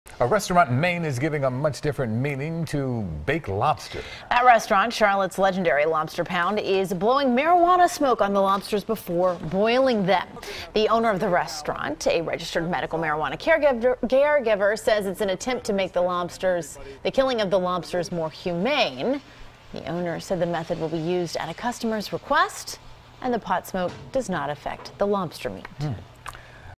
Maine Restaurant Sedates Lobsters With Marijuana Before Cooking Them
A Maine restaurant is using marijuana to give lobsters a high before they’re cooked.